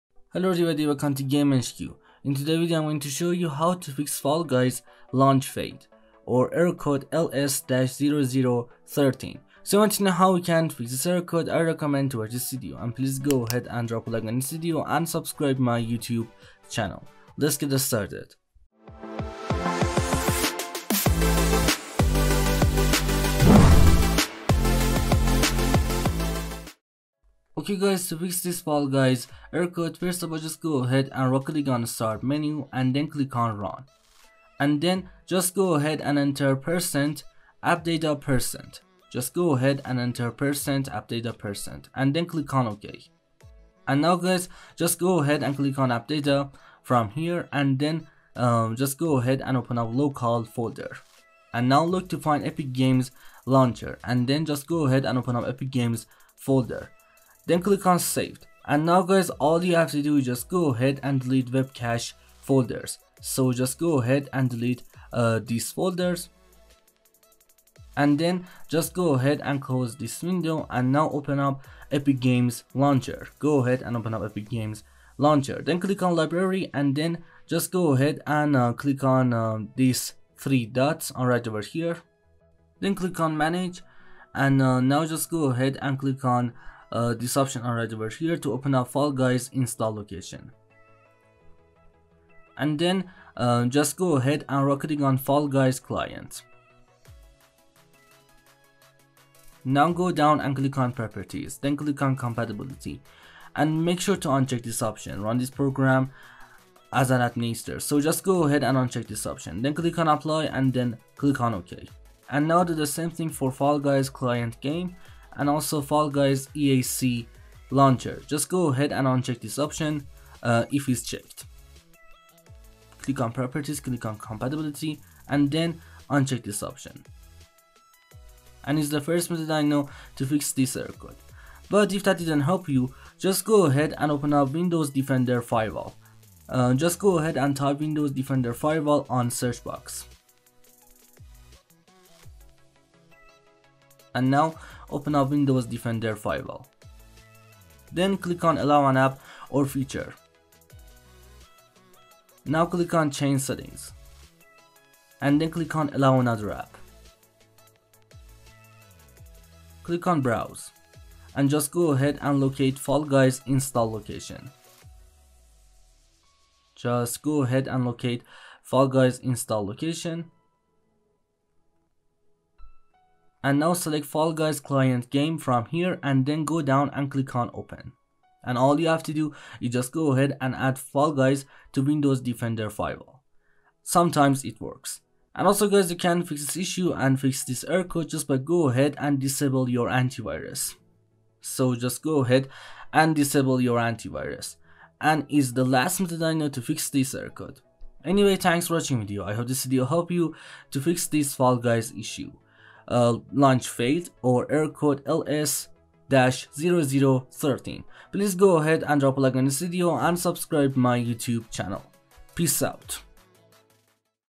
How To Fix Fall Guys Error Code LS_0013 | Fall Guys Launch Failed Fix
Hey everybody in this video I'm showing you How To Fix Fall Guys Error Code LS_0013 | Fall Guys Launch Failed Fix. so if you want to know how to fix fall guys LS-0013 just watch this video.